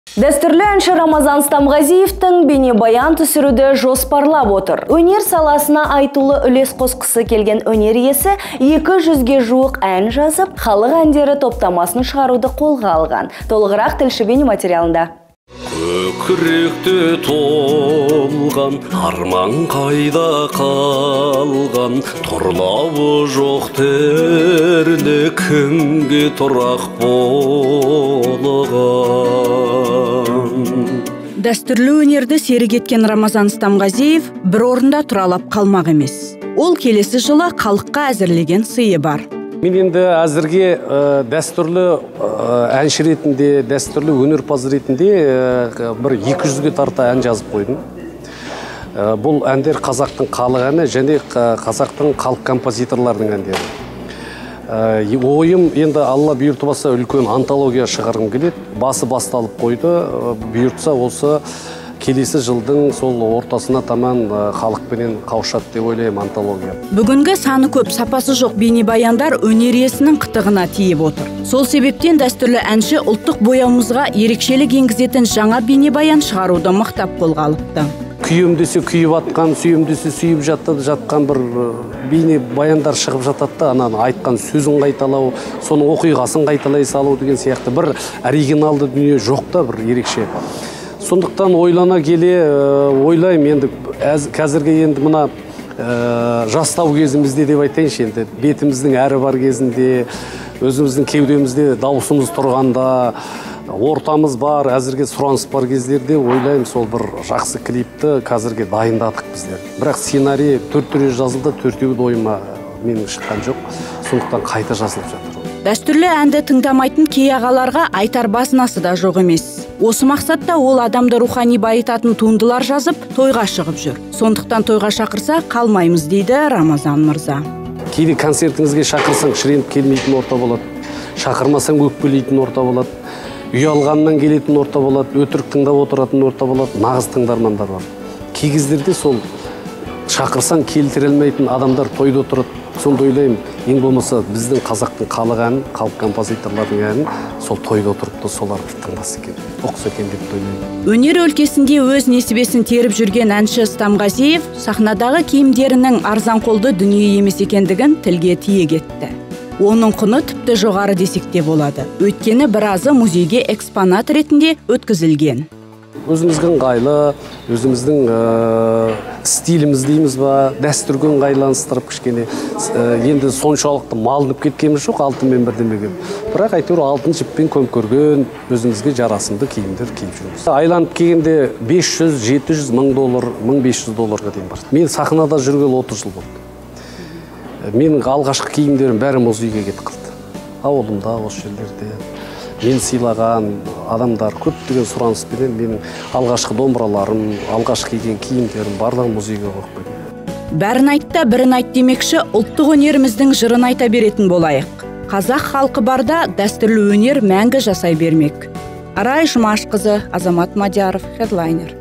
Рамазан Стамғазиевтің xалыққа ұсынар сыйы қандай?
Дәстүрлі әнші Рамазан Стамғазиев тың бейнебаян түсіруді жоспарлап отыр. Өнер саласына айтулы үлес қосқысы келген өнер иесі 200-ге жуық ән жазып, xалық әндері топтамасын шығаруды қолға алған. Толығырақ тілші бейнематериалында.